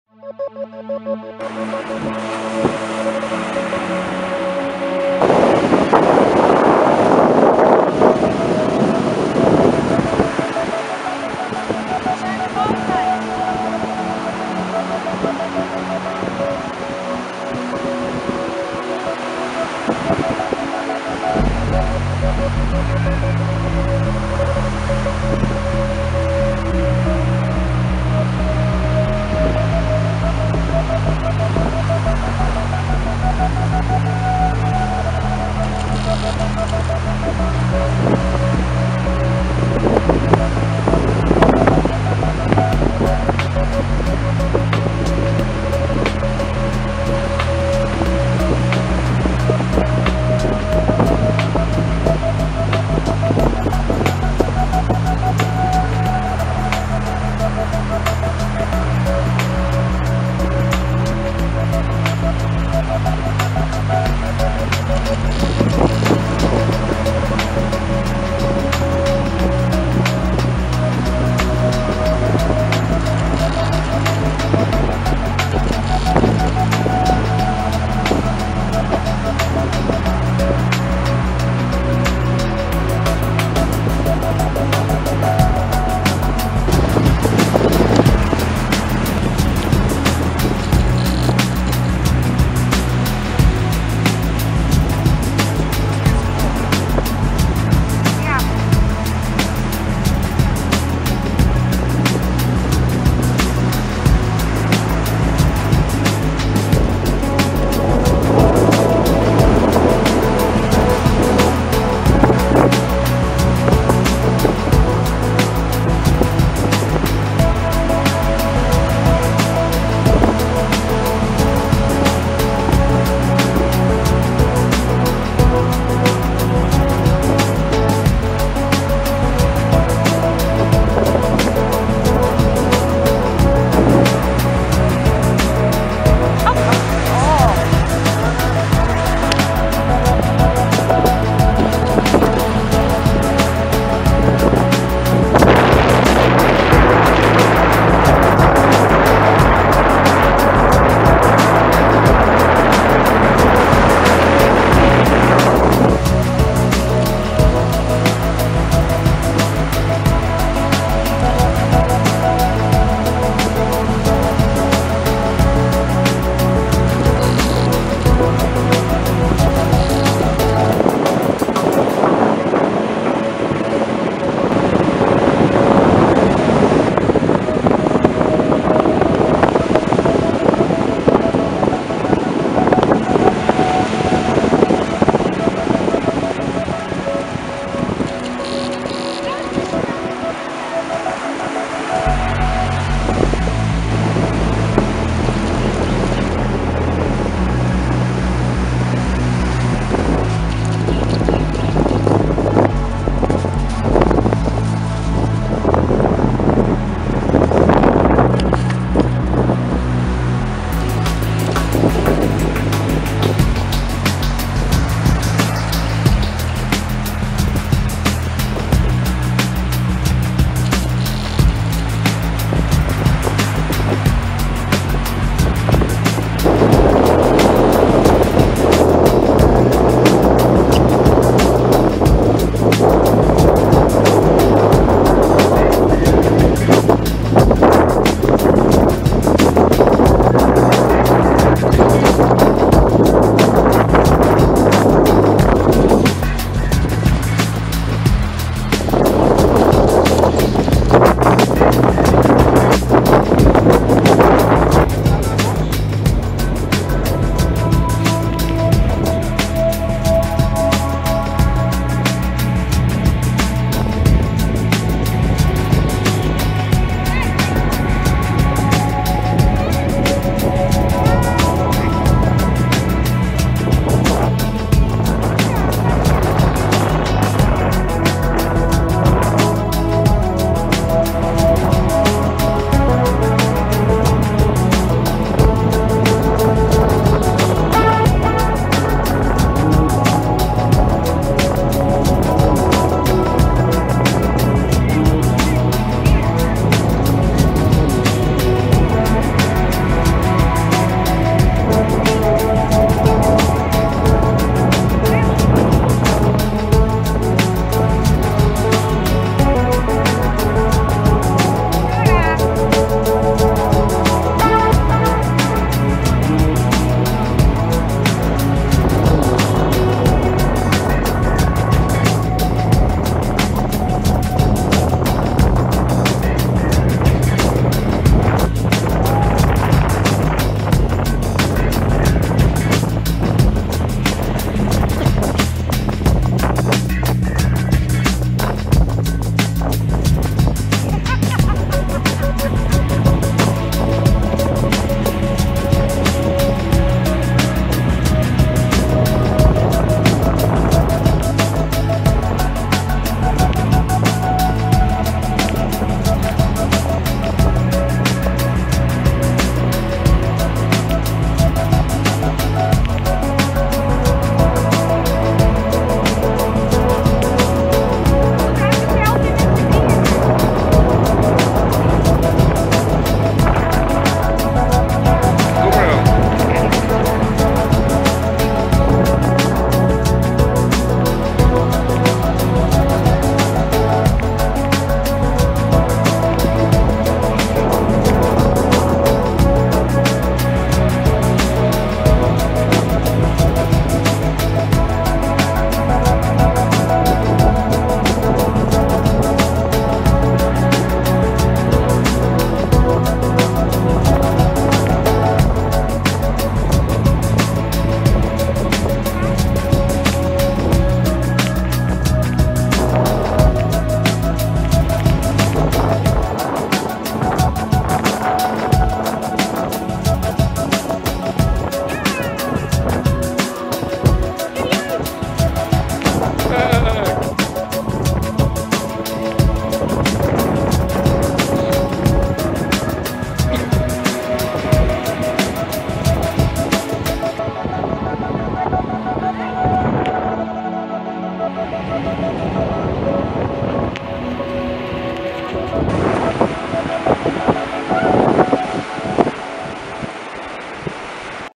dogs Sara, Lotje & Charlie. Zandvoort beach
great weather. december 19. 2015